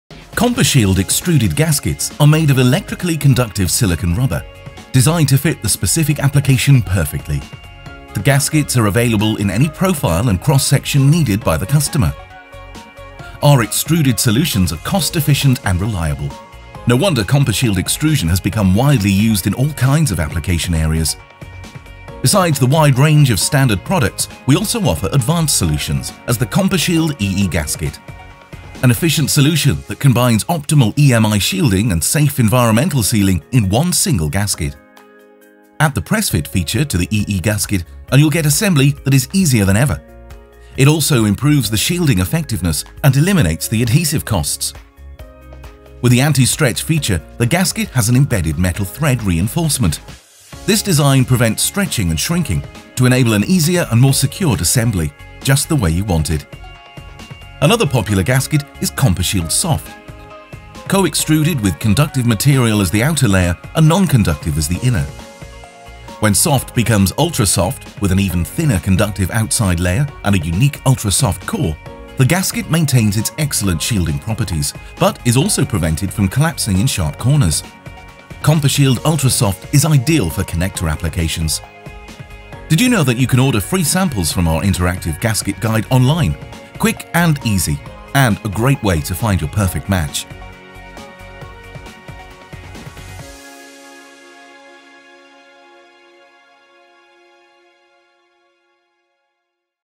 EMI Shielding Solutions - Compashield extrusion
Extruded gaskets from Nolato Silikonteknik are widely used in military and telecommunication applications. Our extrusion process involves gaskets of electrically-conductive silicone rubber being string-injected into the preferred shape and with the cross-section required by the customer. The cross-section can be rectangular, solid or hollow in variuous shapes and dimensions.